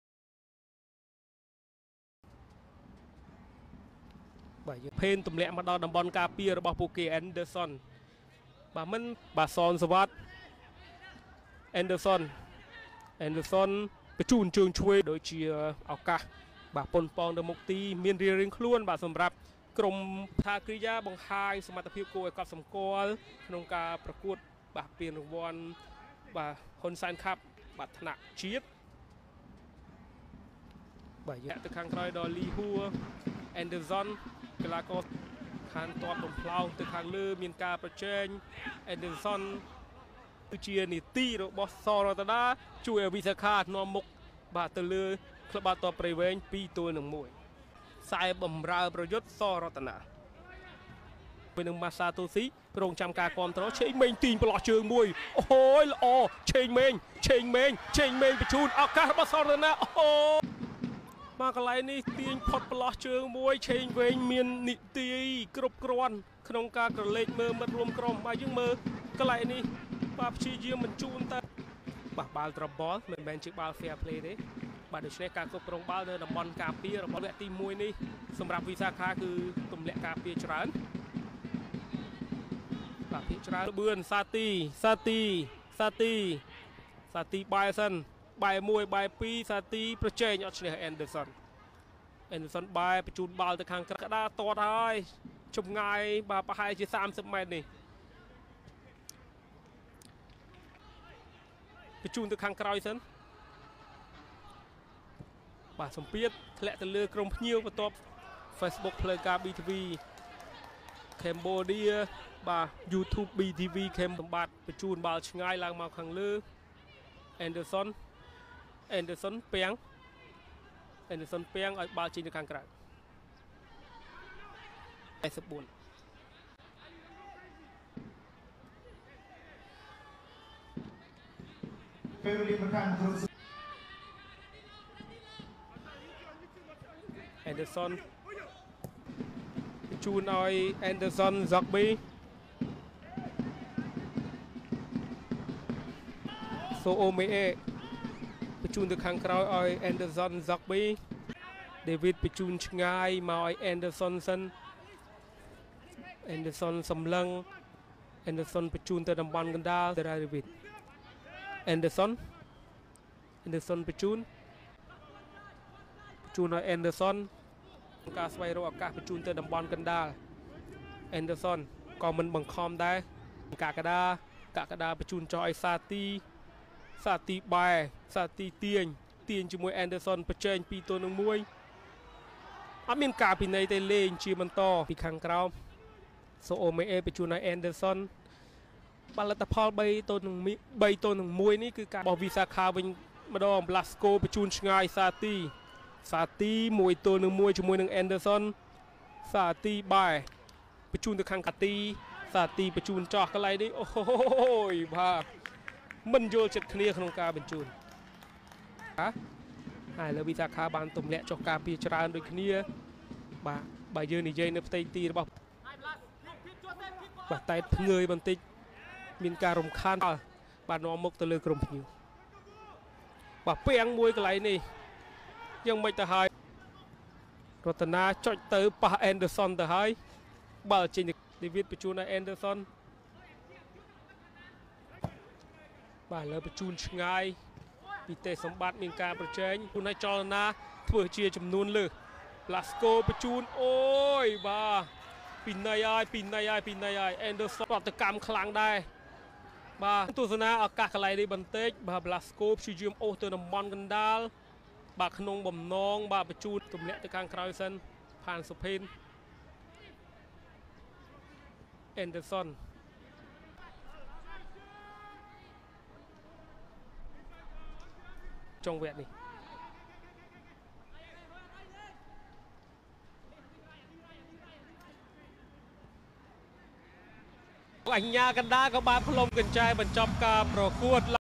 Anderson Zogbe (77) Vs Visakha FC• Anderson Did Hard Work In Prey Veng FC
Personal Highlight For Anderson Zogbe 77, Defender Prey Veng FC in Cambodian Premier League.